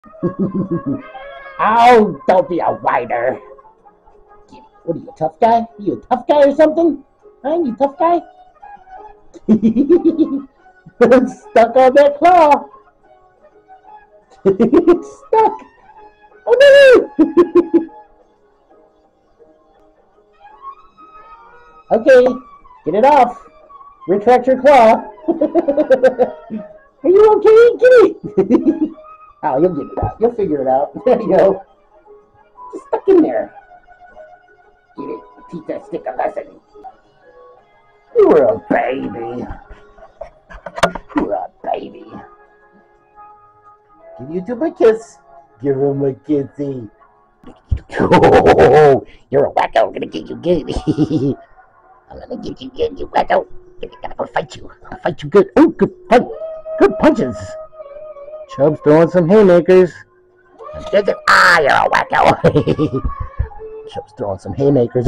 Chubbs the cat. Most famous in 2024.
Chubbs has a weapons malfunction, assures Youtubers he's OK, then gets revenge!